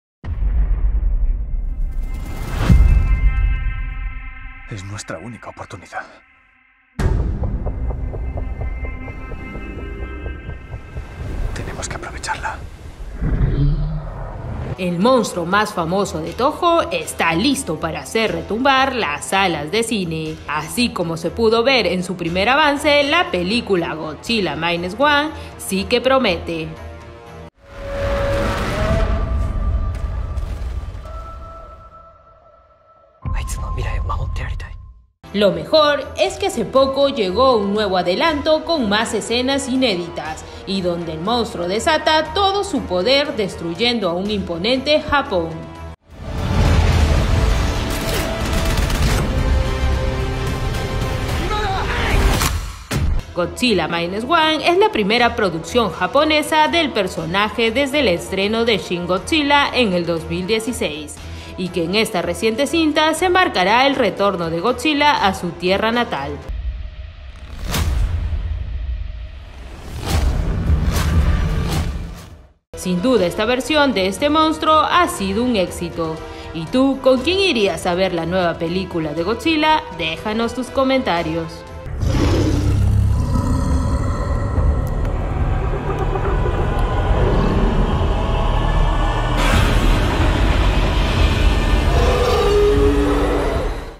CINE| "GODZILLA MINUS ONE" REGRESA A LAS SALAS DE CINES
¡El monstruo más famoso de Toho está listo para hacer retumbar las salas de cine!. Así como se pudo ver en su primer avance la película, ‘Godzilla Minus One’. Sí que promete.